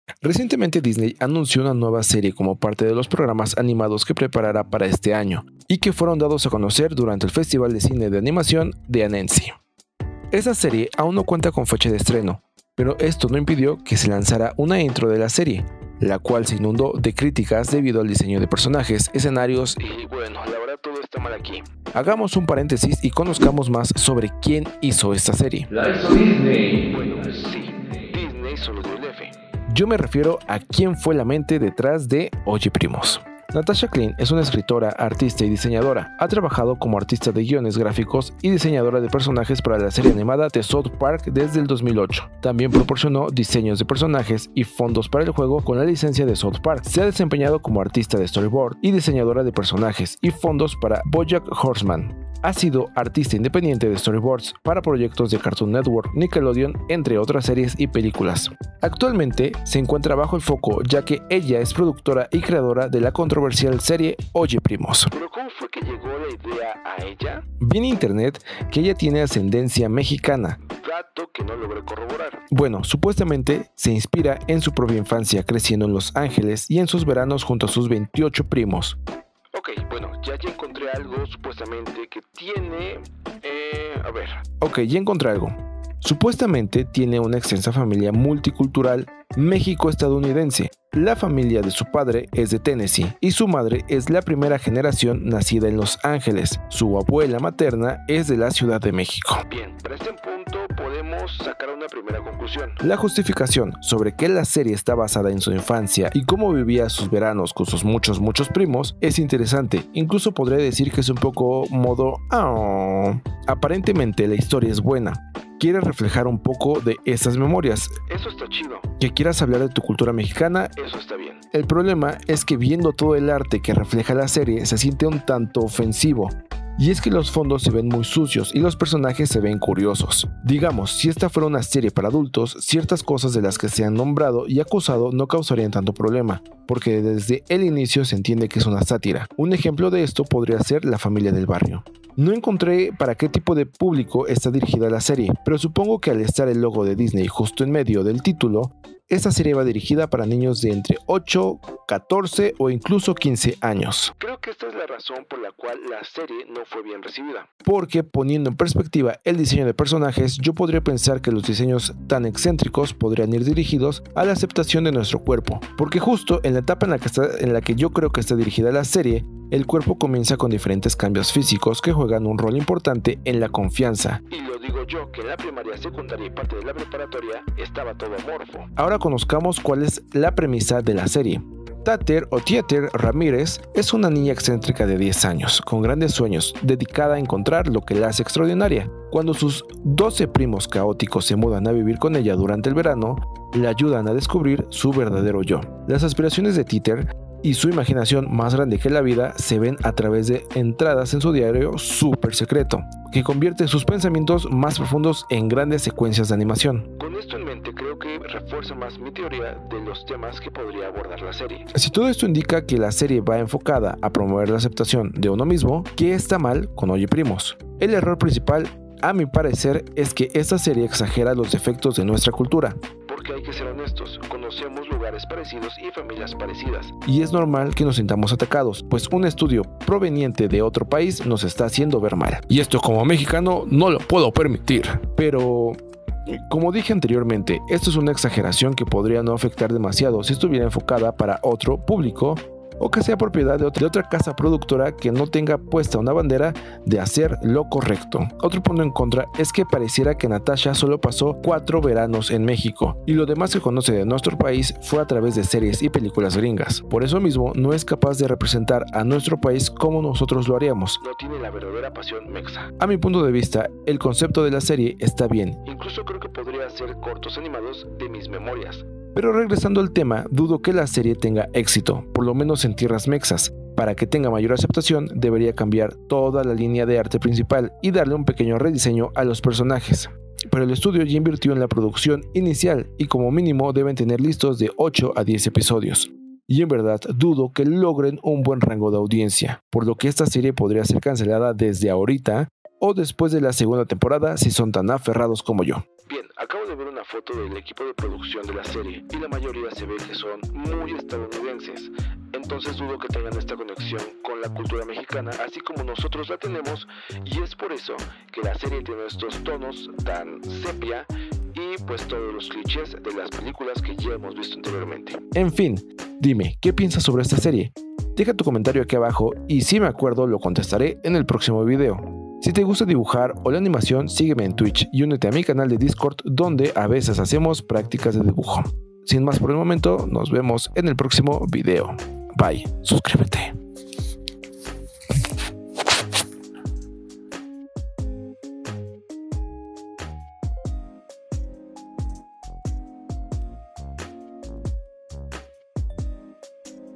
¿que está MAL con OYE PRIMOS? LA NUEVA SERIE DE DISNEY
oyeprimos la serie vino a tocar fibras sensibles para nosotros los mexicanos, y en este video doy la opinión que nadie me die me dio.
Terminando de editar el video desabrí más cosas, pero eso lo dejare para una posible parte 2